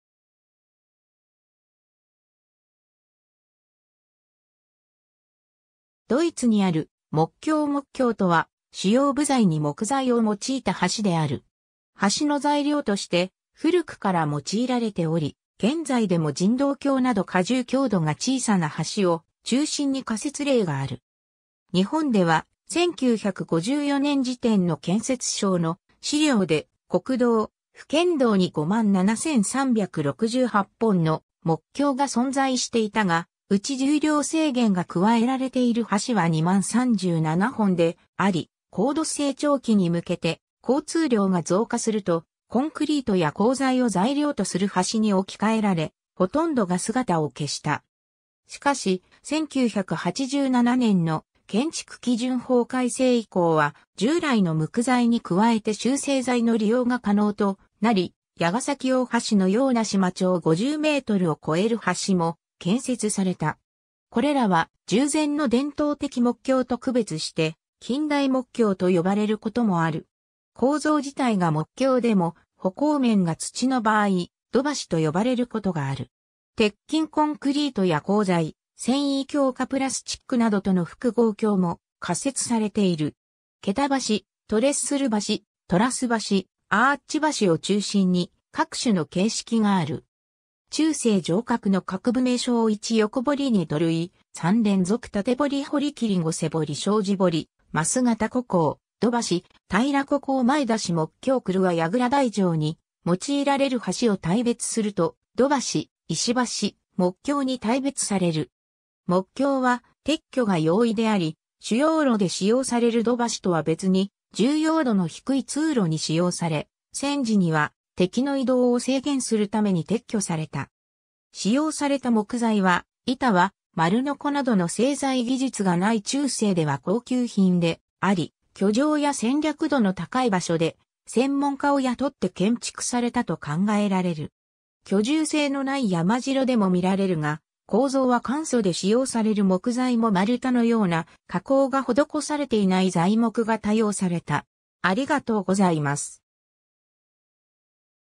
木橋
ドイツにある木橋 木橋（もくきょう）とは主要部材に木材を用いた橋である。
 橋の材料として古くから用いられており、現在でも人道橋など荷重強度が小さな橋を中心に架設例がある。
日本では、1954年時点の建設省の資料で国道、府県道に57,368本の木橋が存在していたが、うち重量制限が加えられている橋は20,037本であり、高度成長期に向けて交通量が増加するとコンクリートや鋼材を材料とする橋に置き換えられ、ほとんどが姿を消した。
しかし、1987年の建築基準法改正以降は、従来の無垢材に加えて集成材の利用が可能となり、矢ケ崎大橋のような支間長50mを越える橋も建設された。
これらは従前の伝統的木橋と区別して「近代木橋」と呼ばれることもある。
構造自体が木橋でも、歩行面が土の場合、土橋と呼ばれることがある。
 鉄筋コンクリートや鋼材、繊維強化プラスチックなどとの複合橋も架設されている。
 桁橋、トレッスル橋、トラス橋、アーチ橋を中心に各種の形式がある。
 中世城郭の各部名称 ①横堀 ②土塁 ③連続竪堀 ④堀切 ⑤畝堀 ⑥障子堀 ⑦枡形虎口 ⑧土橋・平虎口 ⑨馬出 ⑩木橋 ⑪曲輪 ⑫櫓台 城に用いられる橋を大別すると、土橋、石橋、木橋に大別される。
木橋は撤去が容易であり、主要路で使用される土橋とは別に重要度の低い通路に使用され、戦時には敵の移動を制限するために撤去された。
使用された木材は、板は丸鋸などの製材技術が無い中世では高級品であり拠城や戦略度の高い場所で、専門家を雇って建築されたと考えられる。
居住性のない山城でも見られるが、構造は簡素で使用される木材も丸太のような加工が施されていない材木が多用された。